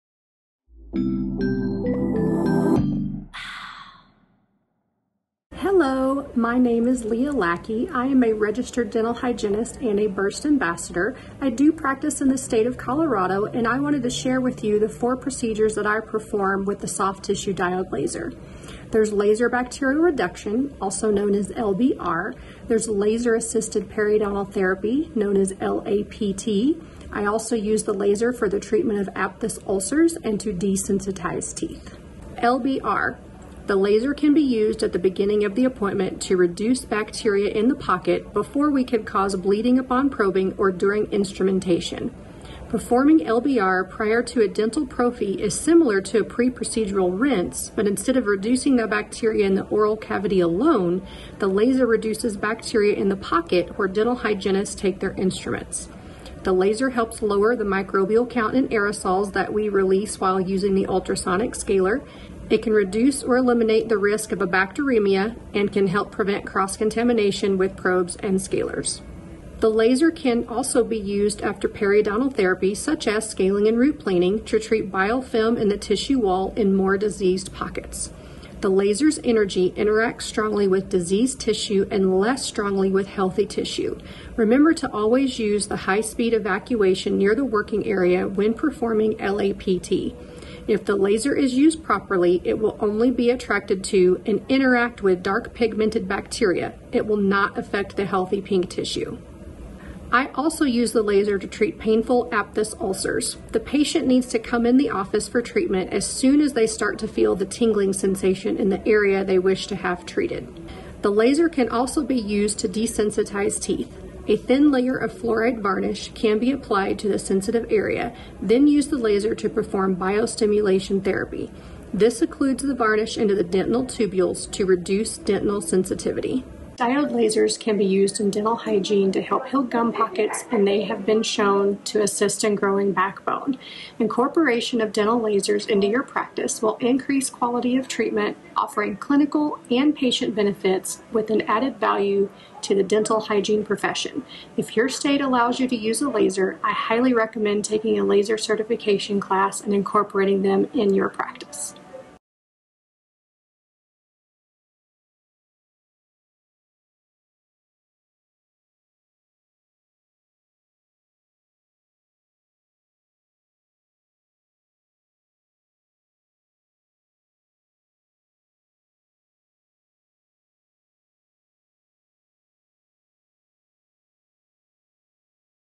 Four ways a dental hygienist can use lasers
Lasers in Dental Hygiene

The word laser is an acronym for the expression "light amplification by stimulated emission of radiation."  In dental hygiene, we use the laser to help reduce the amount of bacteria in the mouth. The four most common procedures used by a Dental Hygienist are Laser Bacterial Reduction (LBR), Laser Assisted Periodontal Therapy (LAPT), the treatment of aphthous ulcers and to desensitize teeth. The laser can be a great tool to aid in keeping your periodontal maintenance patients in remission, it can reduce the bacteria that causes gingivitis and it can keep your prophy patients in a healthy state and reduce their sensitivity.  If your state practice act allows you to use a laser, I highly recommend incorporating it into your practice. Your patients will love the results!
-Leah